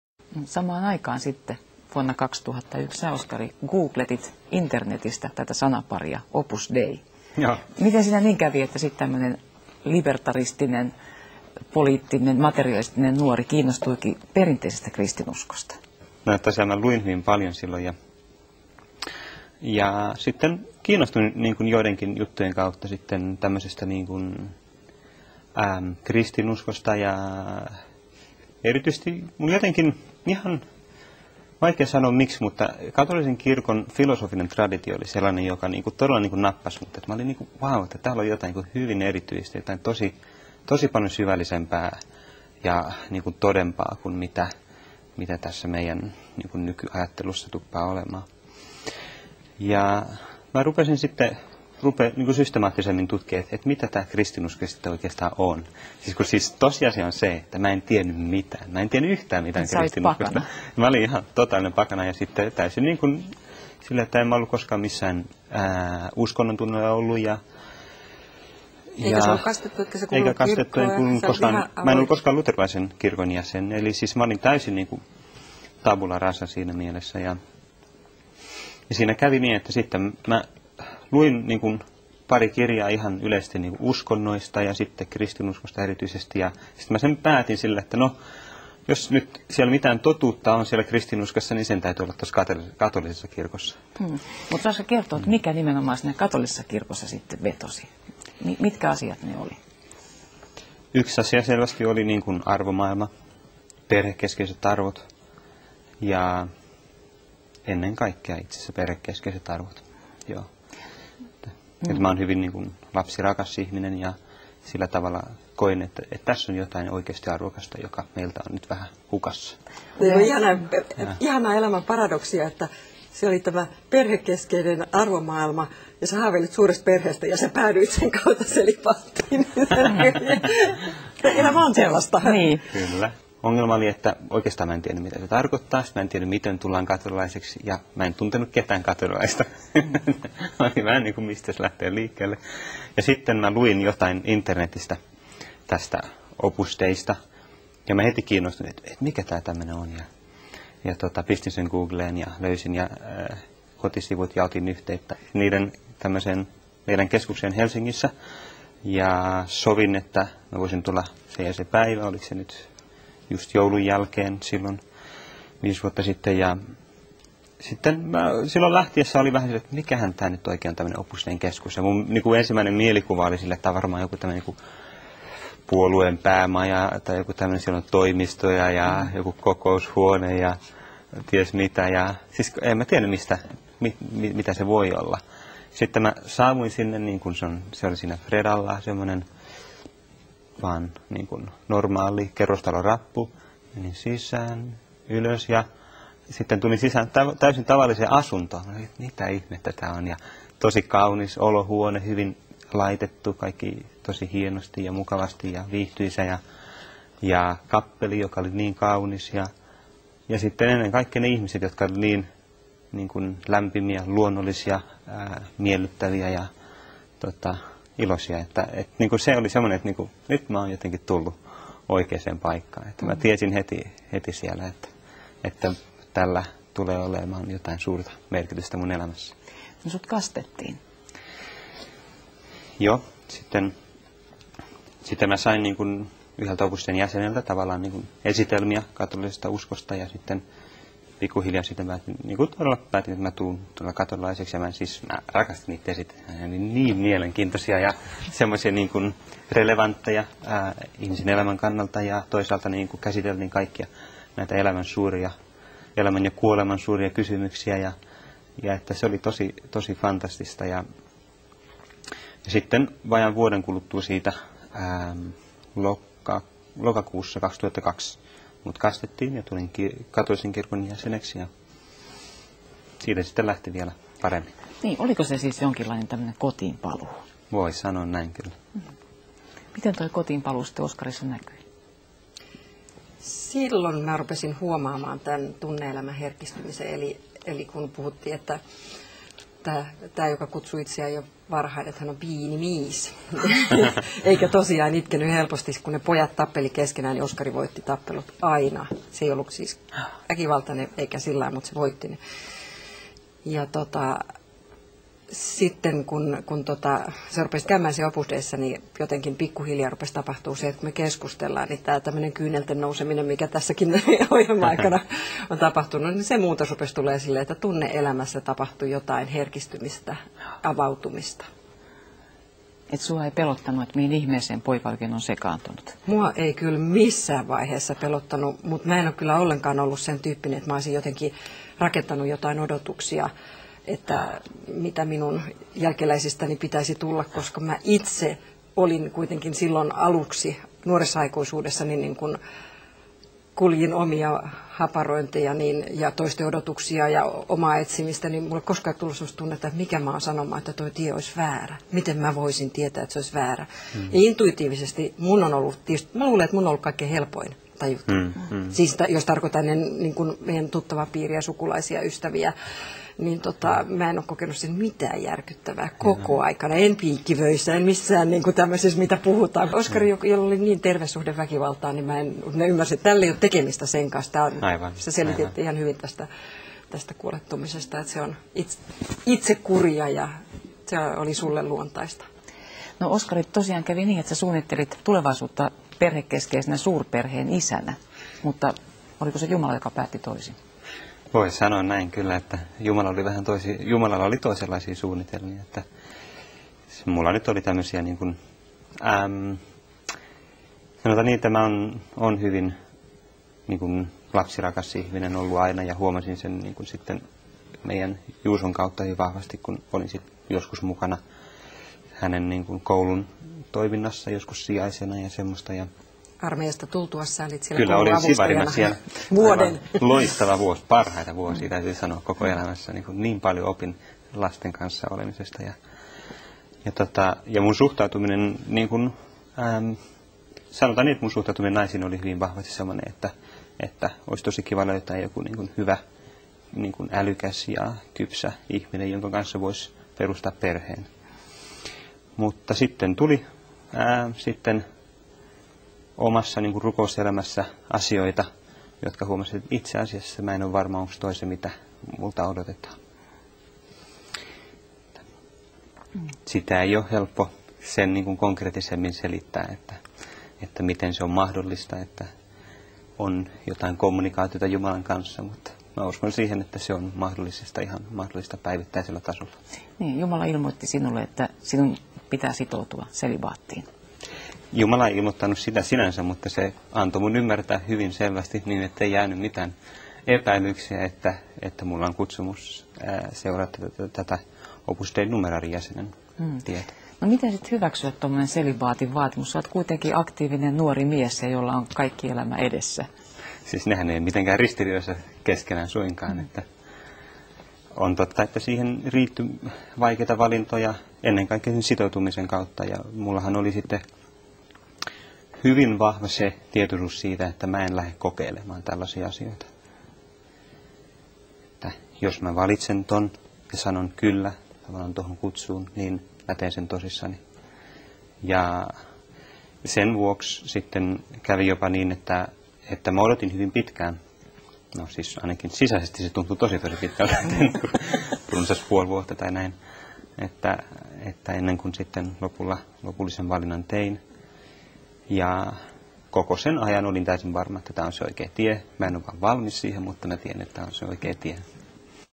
Oskari: De Google a la entrega a Dios
Oskari es un finlandés que en 2001 comenzó a interesarse por la fe cristiana. Ahora, es numerario del Opus Dei. En esta entrevista de televisión explica por qué vive el celibato y cómo ha recorrido este camino.